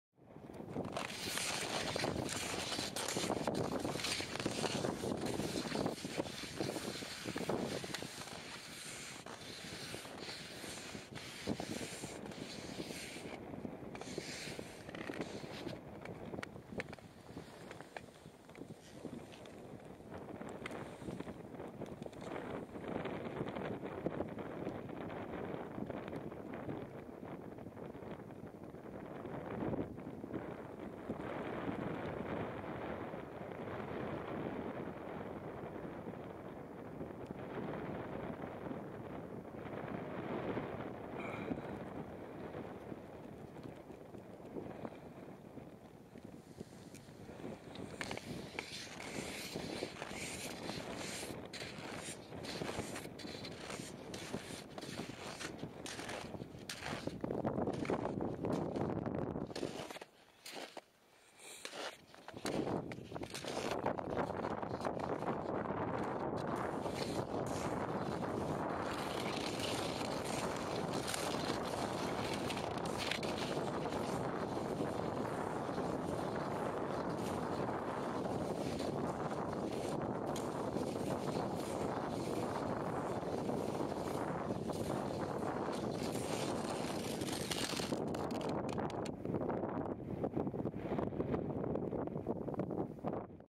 Hiihtoretki Puskuriin / Crossing Puskuri Strait
Hiihtäen Preiviikinlahden Puskurin saareen uusia jäitä pitkin 2/1/2010. Osa I. With skies on new ice to Puskuri isle in Gulf of Bothnia 2/1/2010. A true Rupu-story by Jan Eerala, Makholma Finland.